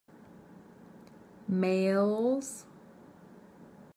How to pronounce the mails/meɪlz/ sound in American IPA
Learn how to pronounce mails .
Học cách phát âm chuẩn của từ mails/meɪlz/ trong tiếng Anh-Mỹ. Để học và luyện tập cách phát âm miễn phí, bạn vui lòng truy cập